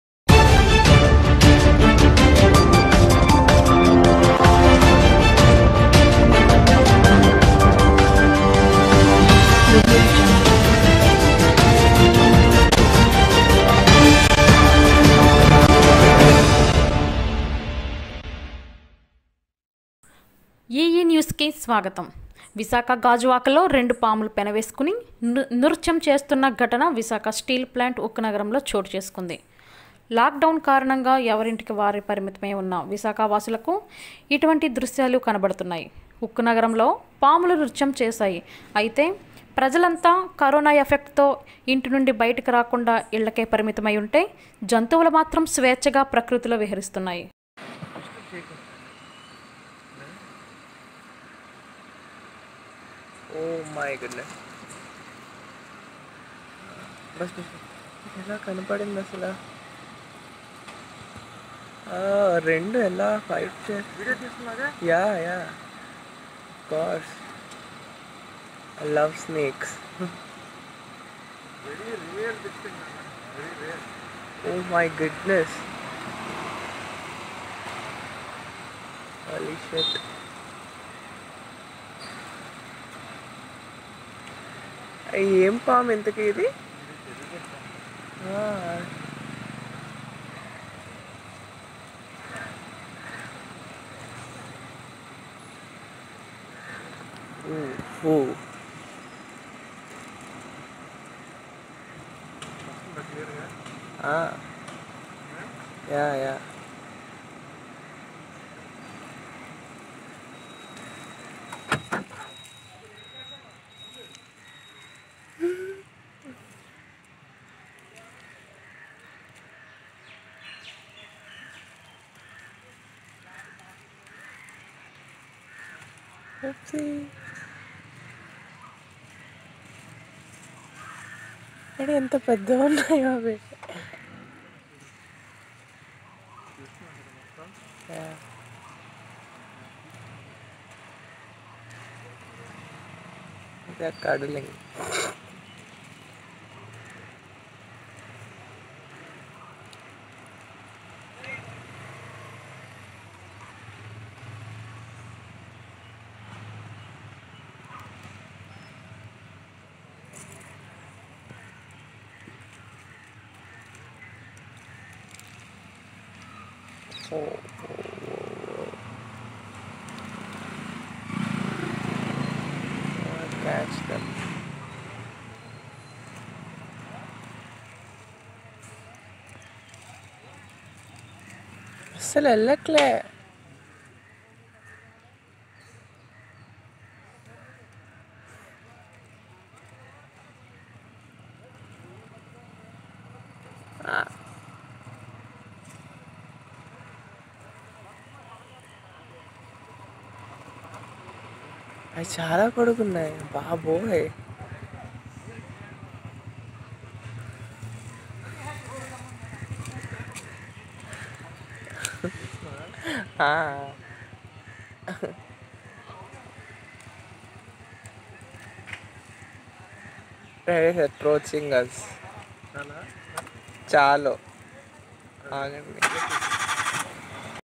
రెండు పాములు నృత్యం.విశాఖ గాజువాక లొ రెండు పాములు పెనువేస్కున్న నృత్యం ఘటన విశాఖ స్టీల్ ప్లాంట్ లో
రెండు పాములు నృత్యం.విశాఖ గాజువాక లొ రెండు పాములు పెనువేస్కున్న నృత్యం ఘటన విశాఖ స్టీల్ ప్లాంట్  ఉక్కునగరం లో